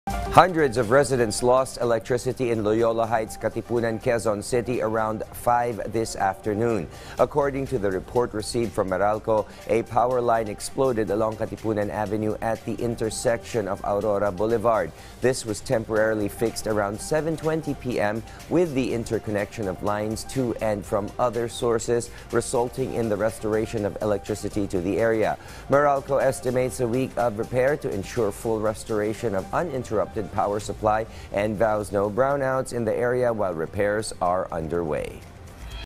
Power line at corner of Katipunan Ave., Aurora Boulevard in QC explodes, leaving hundreds without...
Power line at corner of Katipunan Ave., Aurora Boulevard in QC explodes, leaving hundreds without electricity